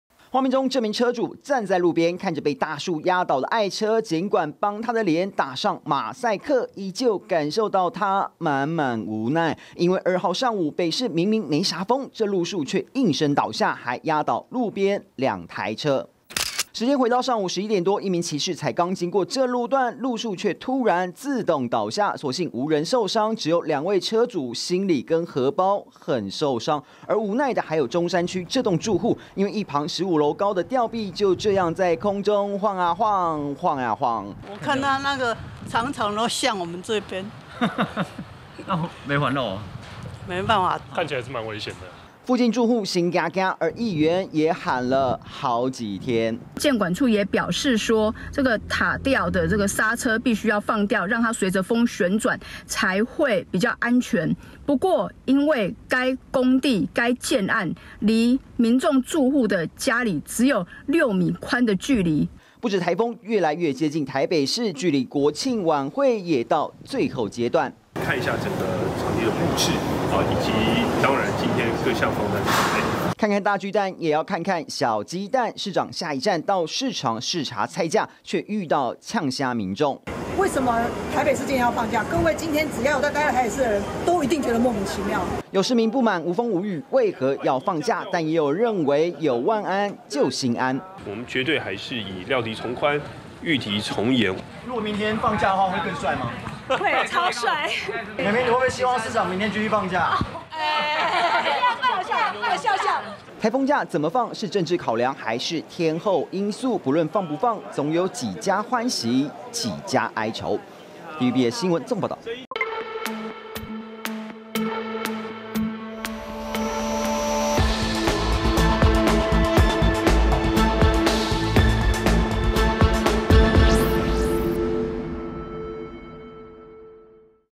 北市風雨增強「進行式」 上午38件路樹倒塌｜TVBS新聞
明天北北基桃是否繼續放颱風假，備受外界關注，台北市長蔣萬安下午視察市場物價時就表示，北市絕對"料敵從寬、禦敵從嚴"，儘管今天(2)大多數市民未感受到颱風威力，但北市光是上午，因強風導致的路樹倒塌災情就多達38起。